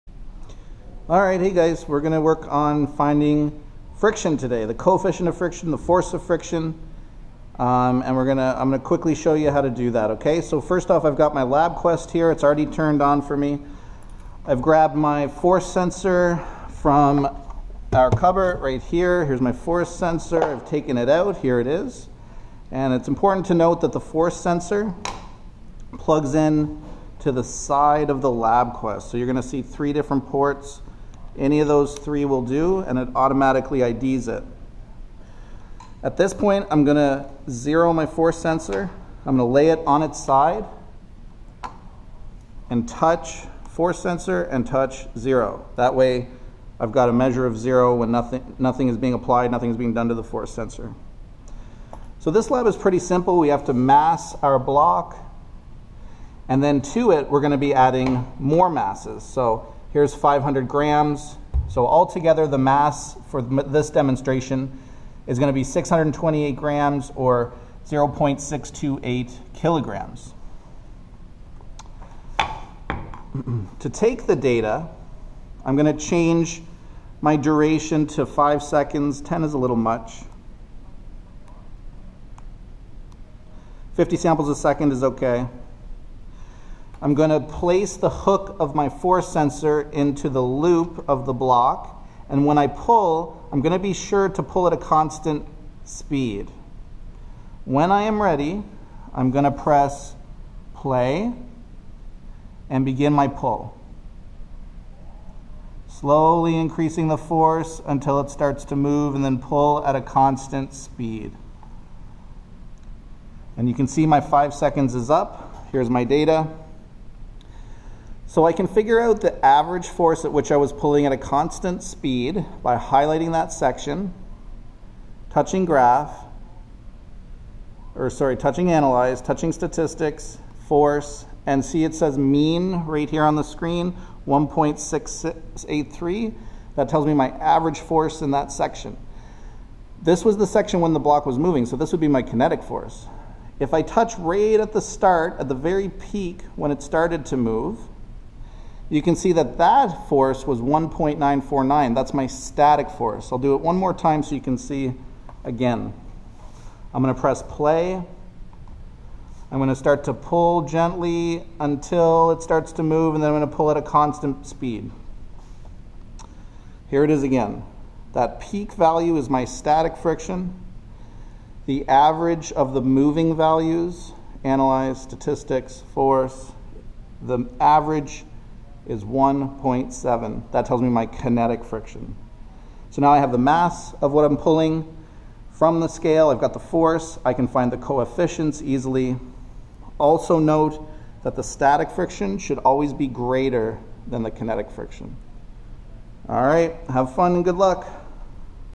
coefficient of friction lab
Quick demonstration of set up for calculating coefficients of friction between a lab bench surface and a block of wood.